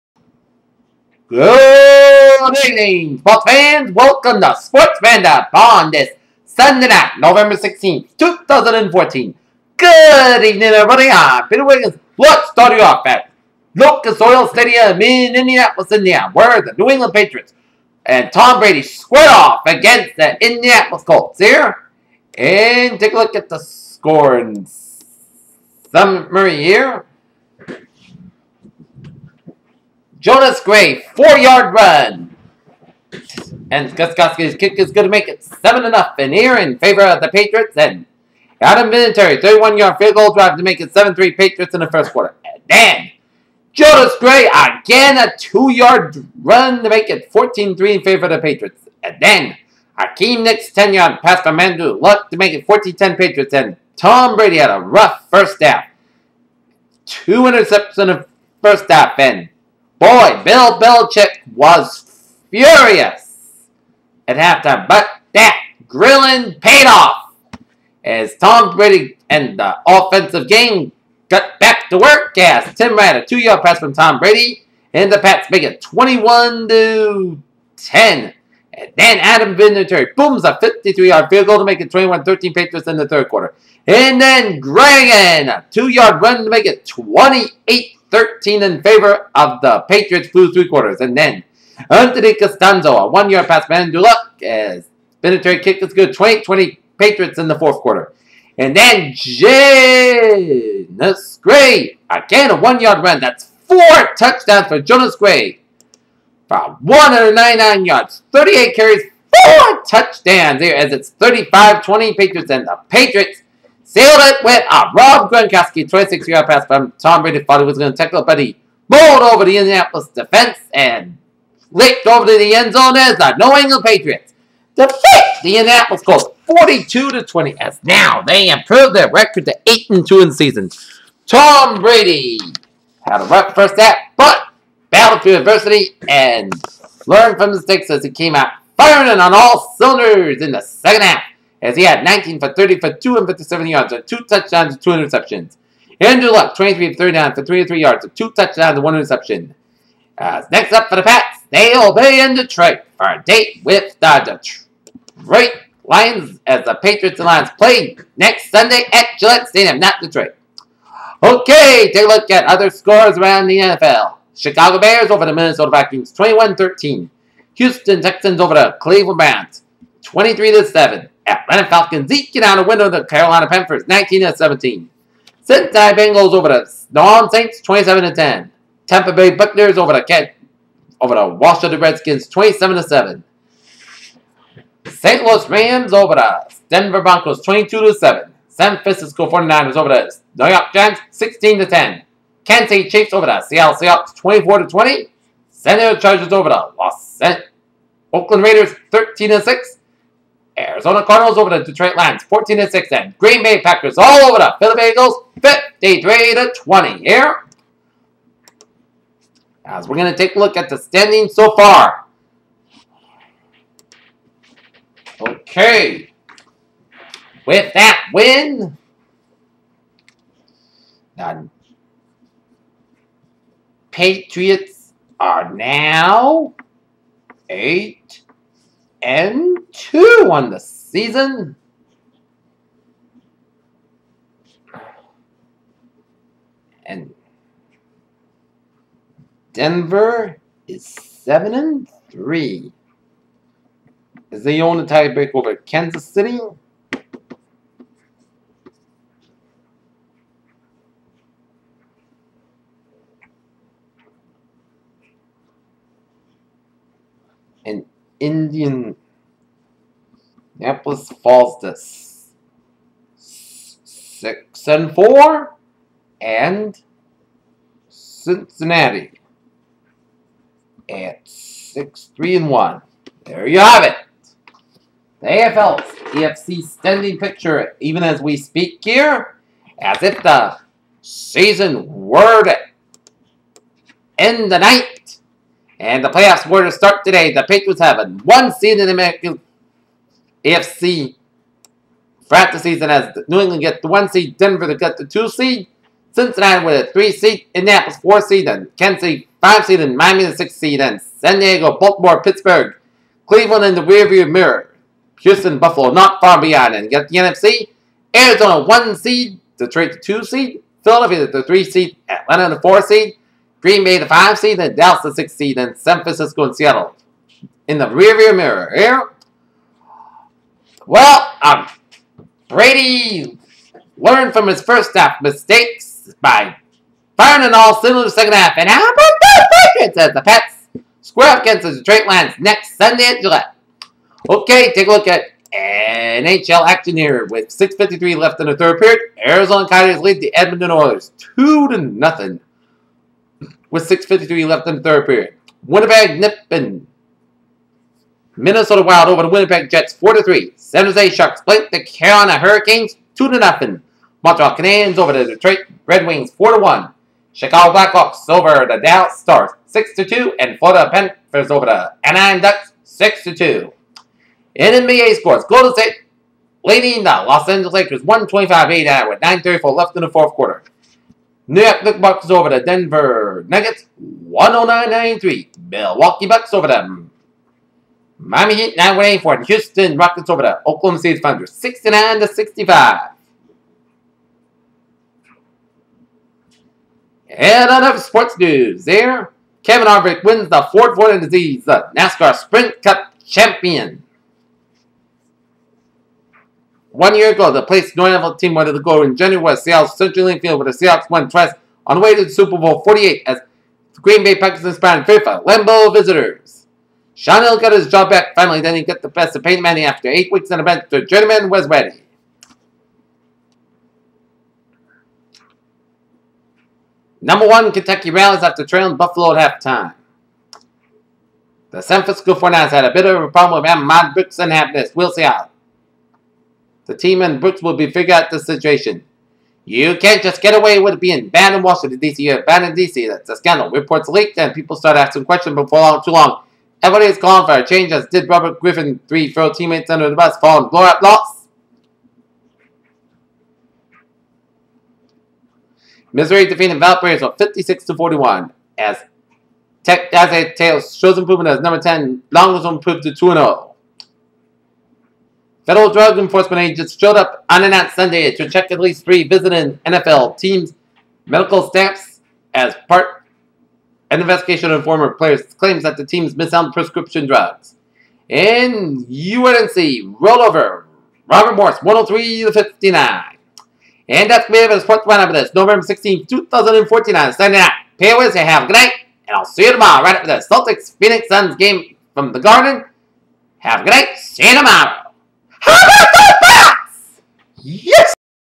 Sports Roundup with Peter J. Wiggins- Sunday November 16, 2014
A summary of tonight's sports action

1. New England Patriots defeat the Indianapolis Colts at Lucas Oil Stadium, 42 to 20 as they improve to 8 and 2 on the season and a look at the NFL Scoreboard

2. NHL Scoreboard

3. NBA Scoreboard

4. Sports News